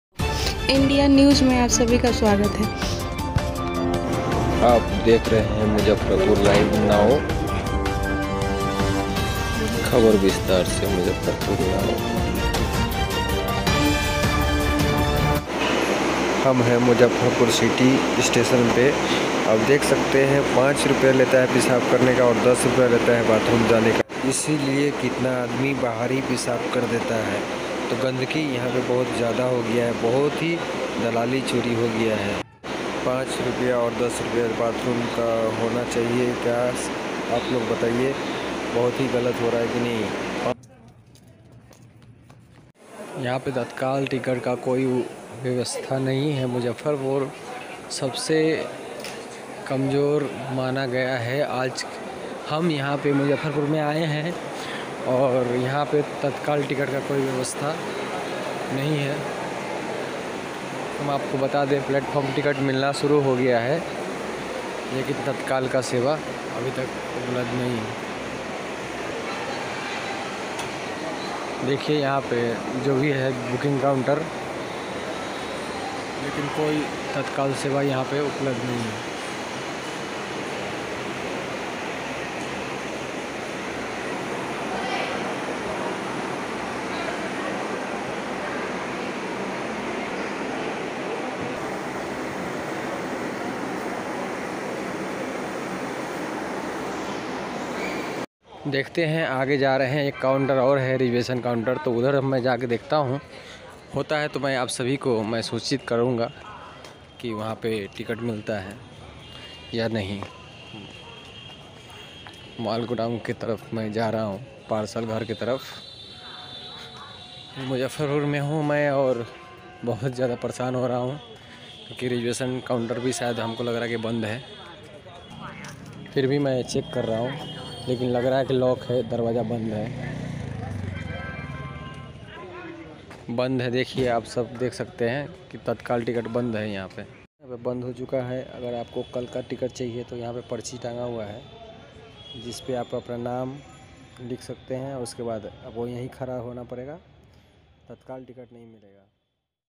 मुजफ्फरपुर रेलवे स्टेशन पर गरीब के जेब होता है खाली पिसाब करने के भी चाज5₹रूपये बाथरूम का 10
गरीब के जेब हूआ खाली

रिजर्वेशन टिकट फिली लाइन लगाना पड़ता है लंबा

बिहार स्थित

बहुत बहुत बुरा हाल है हमारे देश की

किसे किसने समझाये